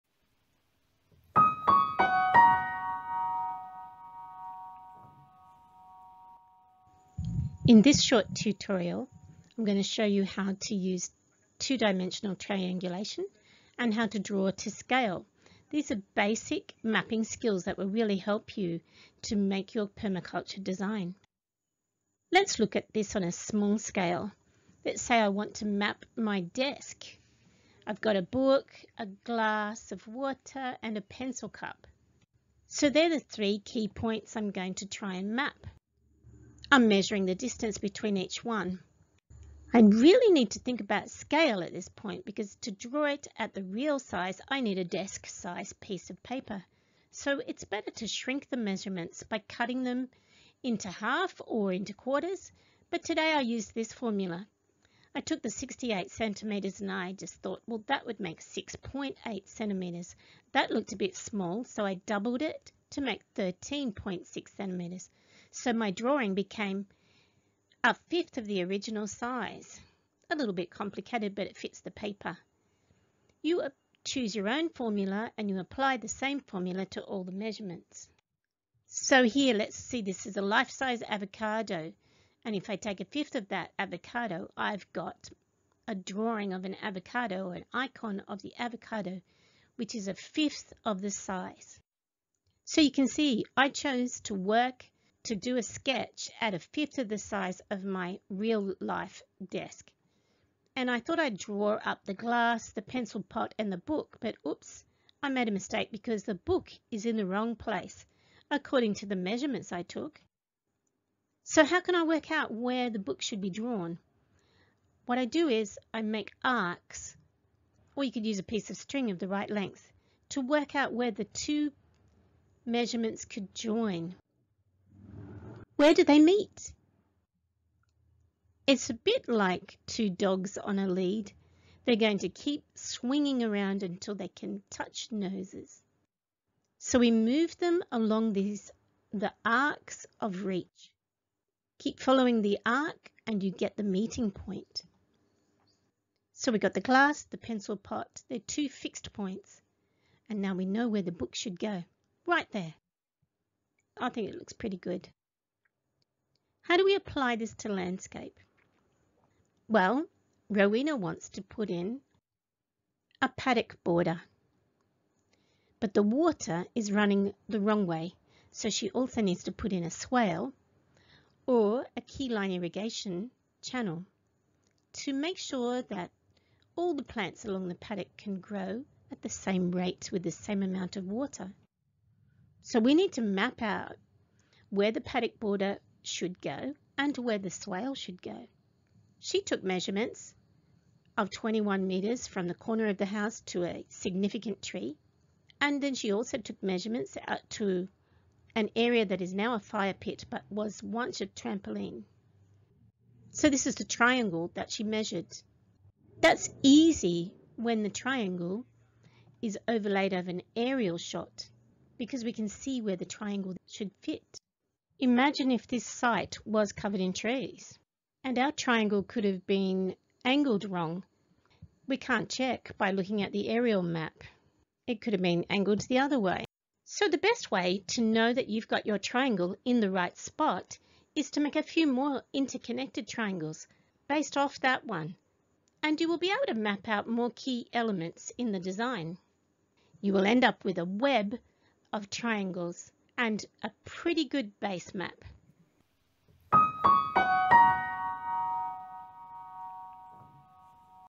Map it with Triangles
2D Triangulation and Drawing to Scale - a beginners guide to mapping an area in preparation for your design plan. We also introduce you to scale and why that helps.